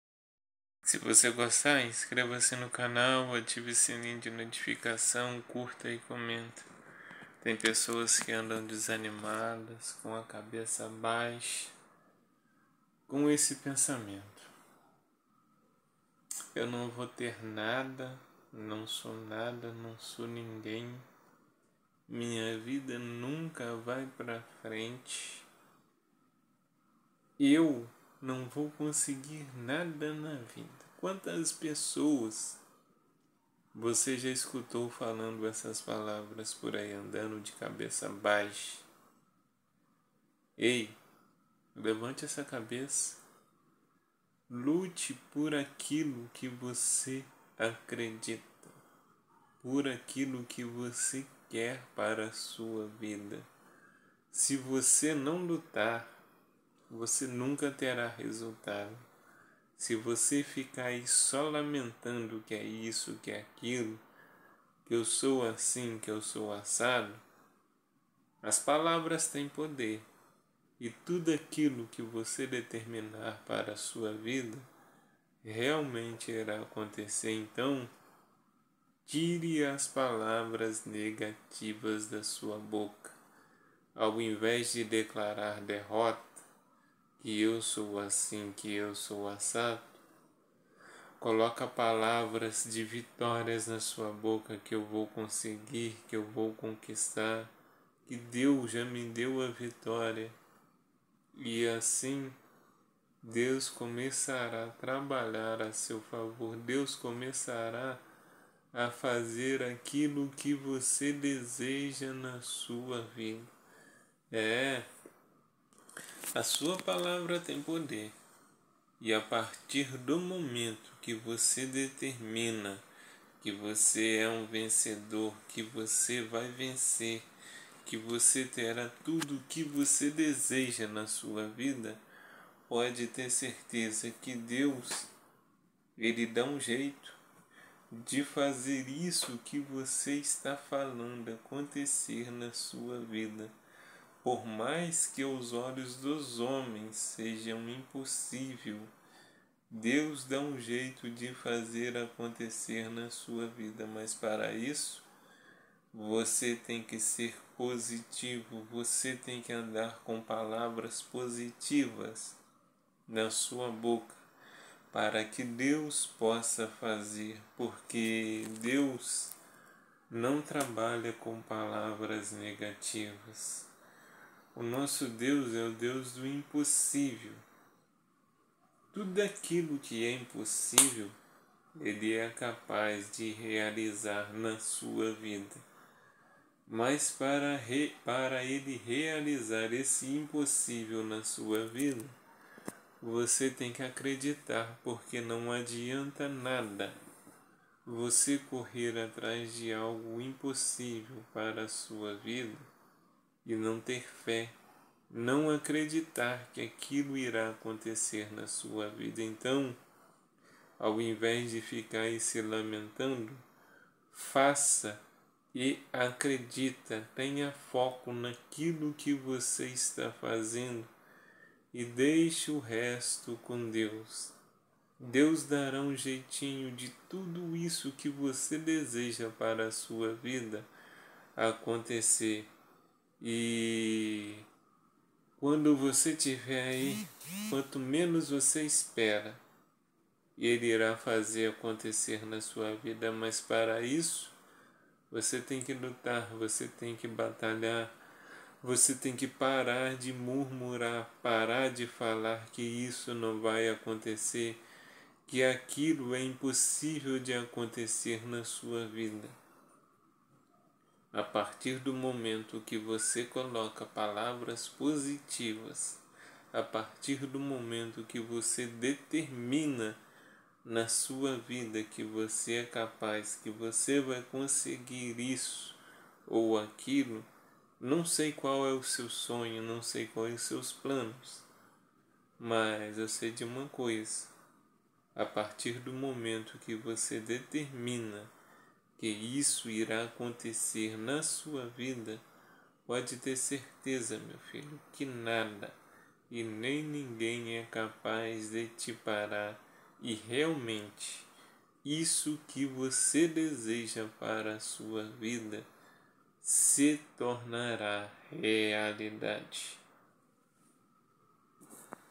Levanta e lute por aquilo que acredita
A partir do momento que você acredita que é capaz de realizar qualquer coisa na sua vida realmente as coisas começam a acontecer, pare de dizer que não é ninguém que não consegue nada na vida, e lute por aquilo que você realmente acredita que Deus dará um jeito disso se tornar realidade na sua vida .